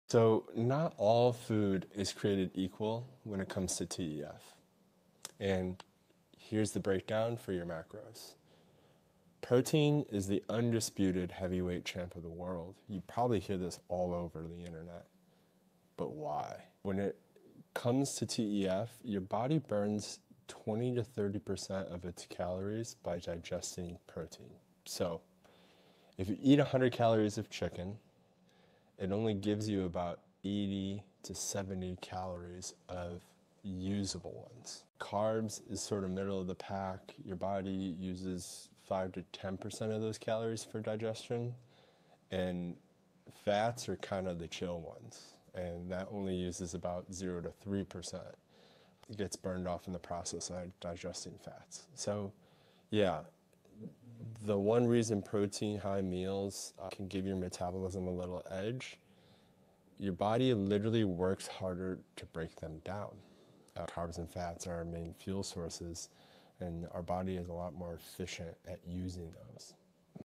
Protein vs. Carbs vs. Fats: TEF Values Breakdown!
Not all foods are created equal when it comes to TEF (Thermic Effect of Food). Protein reigns supreme, burning 20-30% of calories during digestion. Carbs use 5-10%, while fats barely register at 0-3%. Protein gives metabolism an edge because the body works harder to break it down.